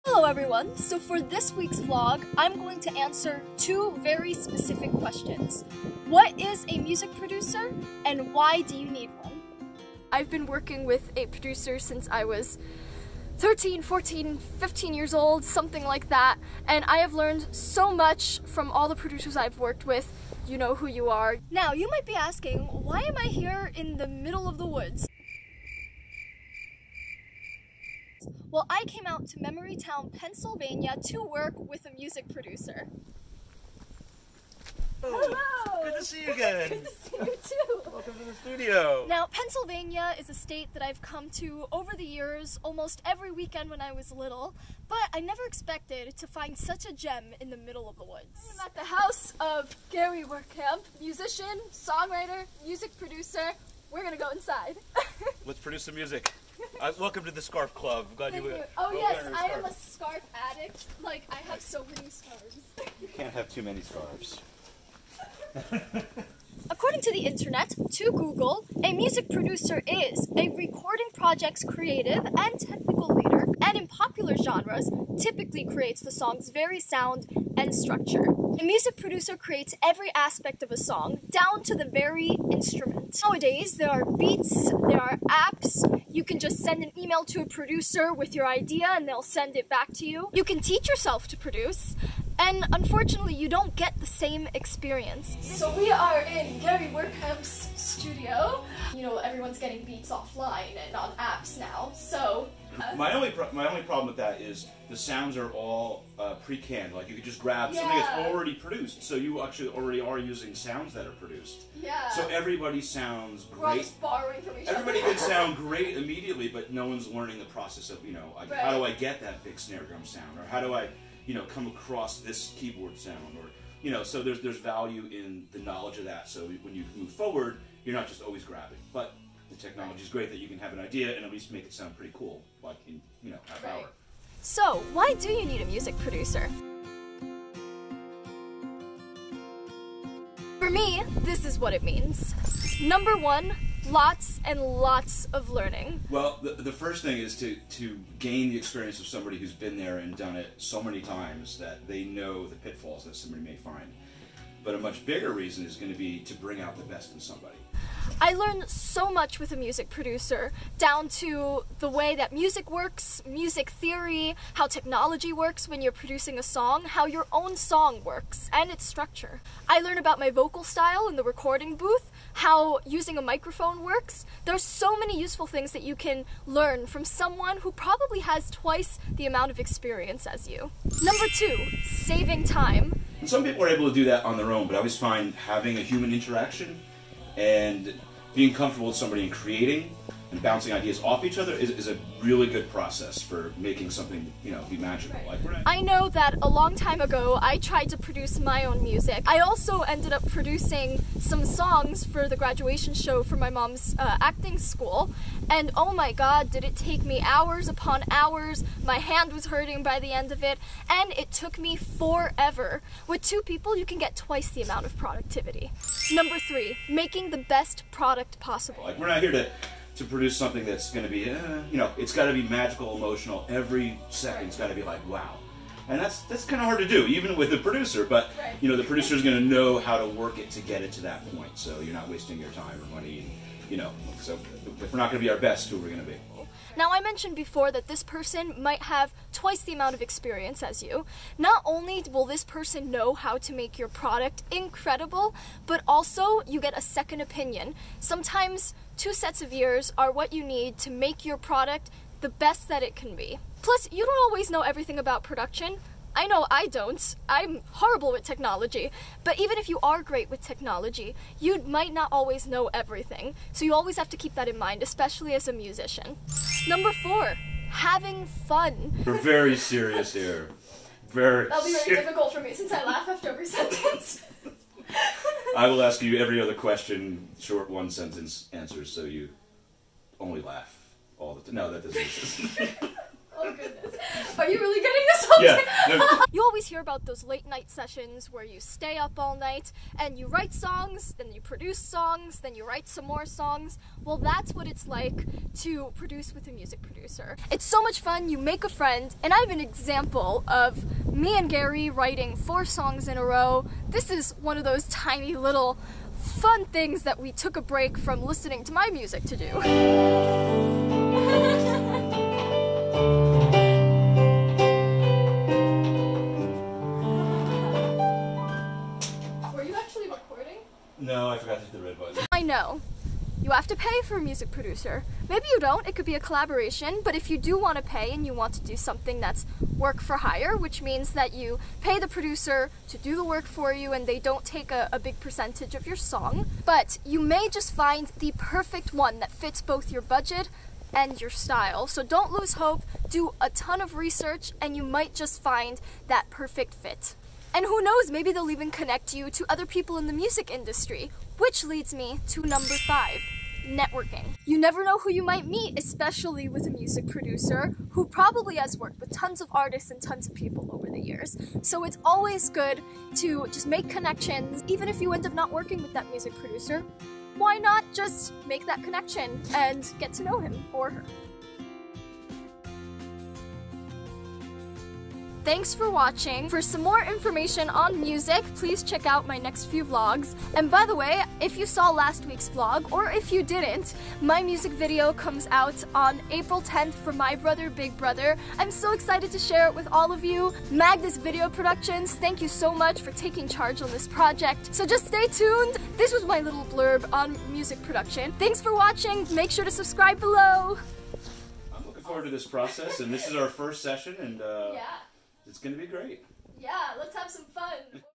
What is a Music Producer? | Top 5 Reasons why you need one!
Have you ever wondered what a music producer is and what they do?  Do you feel like you even need one?  Well, thanks for asking!  In this video, I’m going to not only answer the question of what a music producer is, but also, give 5 great reasons as why you would absolutely benefit by hiring one!

Contents
0:00 Intro
1:07 What is a music producer?
2:14 Why do you need a music producer?
2:24 Reason #1
3:01 Reason #2
3:36 Reason #3
4:27 Reason #4
5:52 Reason #5
6:15 Conclusion